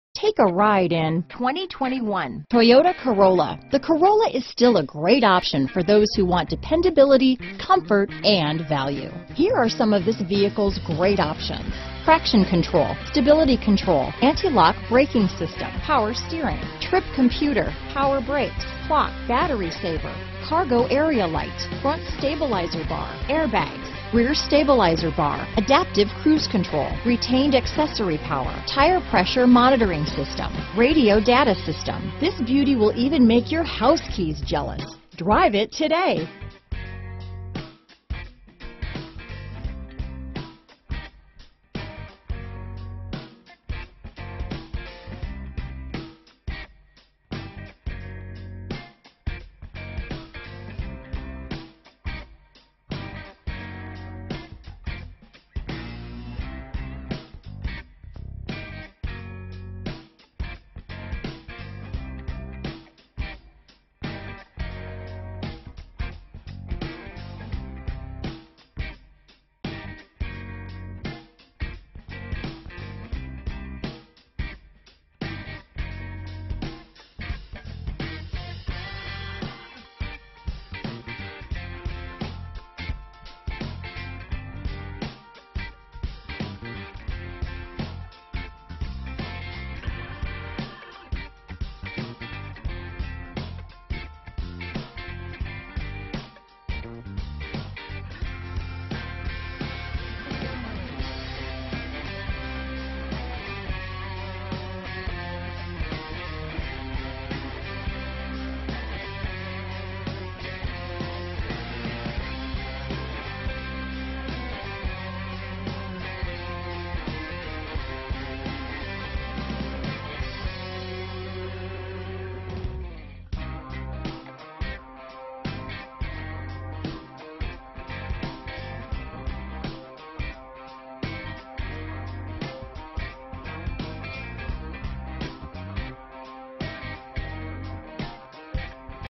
2021 Toyota Corolla Walk Around
Super White New 2021 Toyota Corolla walk around available in Carson City, Nevada at Carson City Toyota. Servicing the Reno, Northern Nevada,  Dayton, Lake Tahoe, NV area.